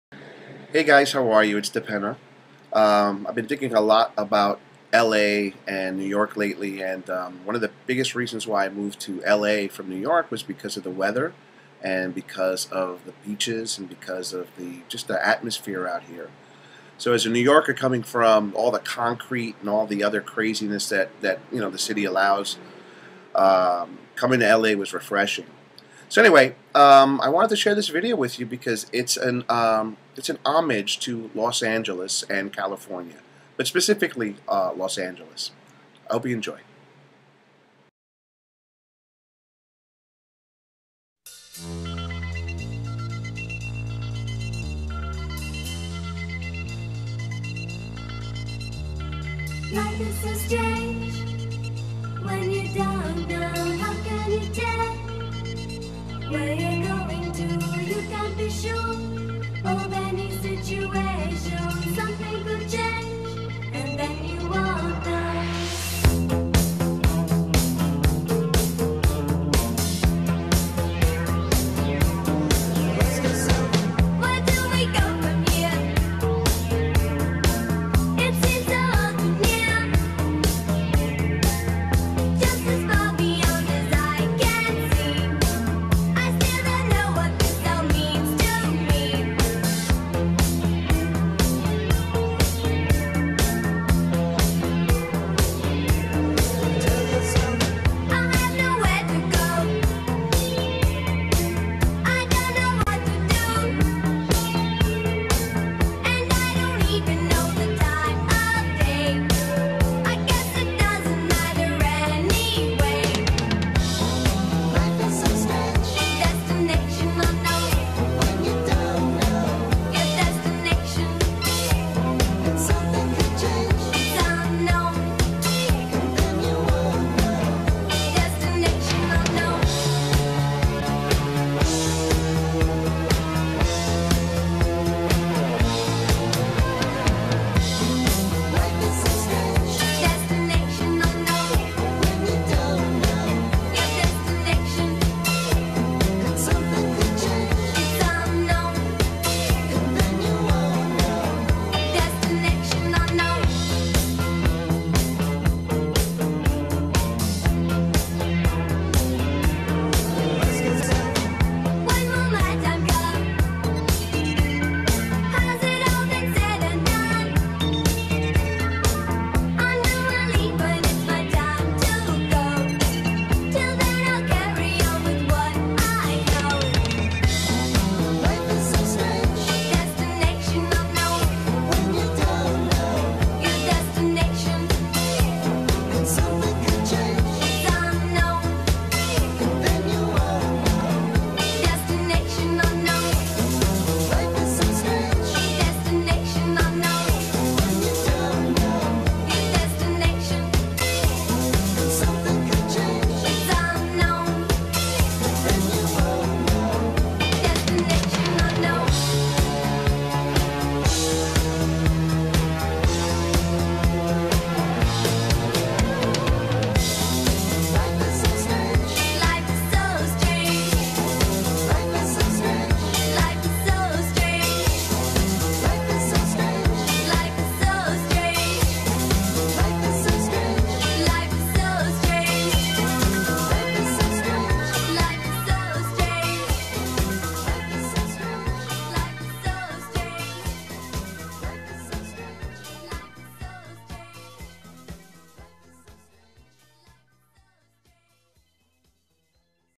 LA trip 1999
My trip to Los Angeles back in 1999.  A working vacation.